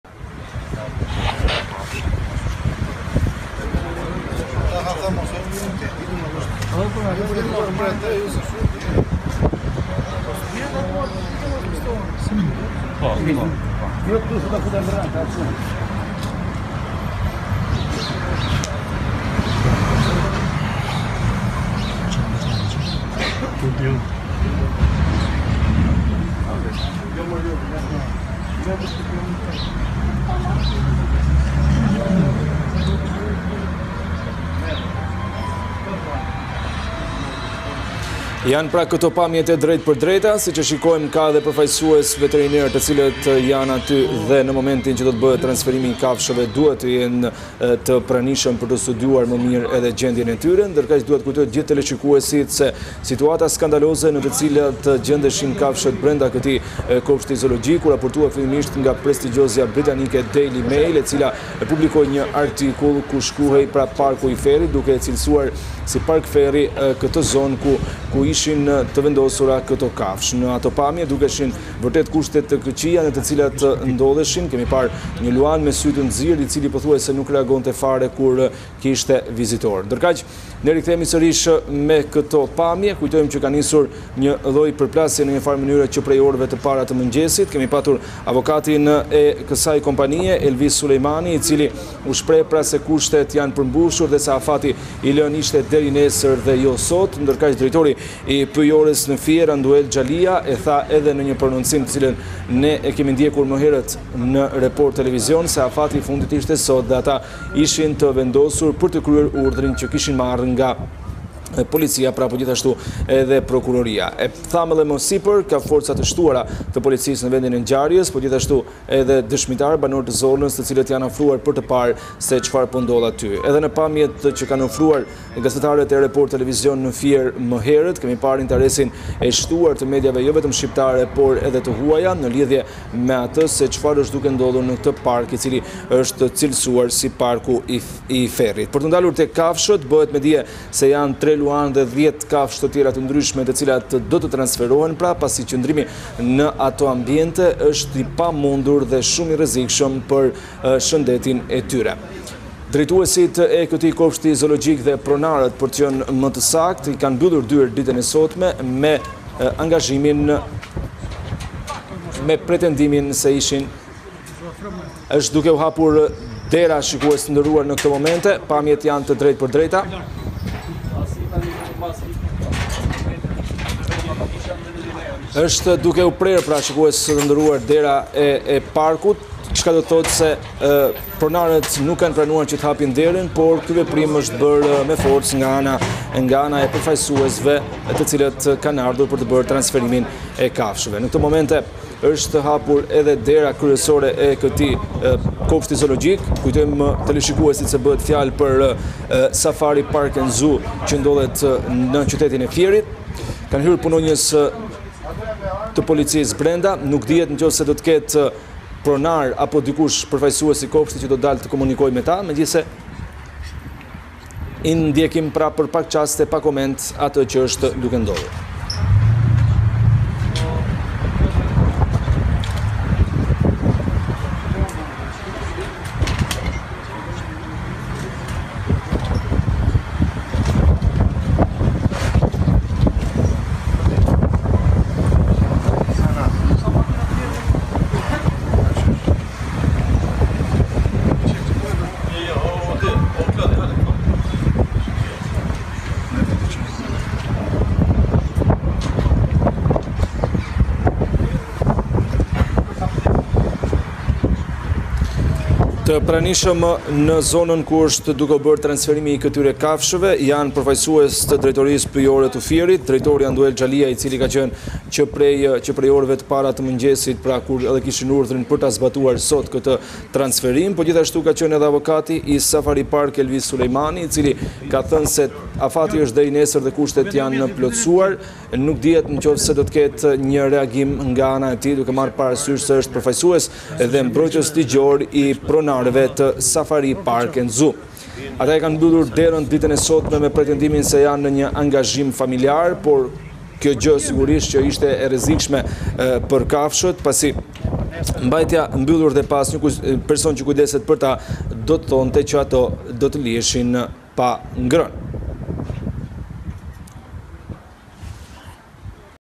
Fier, inspektorët hapin me forcë derën e "Safari Park', nis operacioni për transferimin e kafshëve
Report Tv ka siguruar pamje nga momentet kur ata kanë hyrë brenda këtij parku, për të bërë të mundur transferimin e 13 kafshëve, mes tyre 4 luanë, drejt Tiranës. Ata kanë prerë me pinca telat e portës hyrëse në kopësht, pasi pronarët e këtij kopështi nuk ndodhen atje, dhe s’kanë lejuar që transferimi i kafshëve të bëhet rregullisht.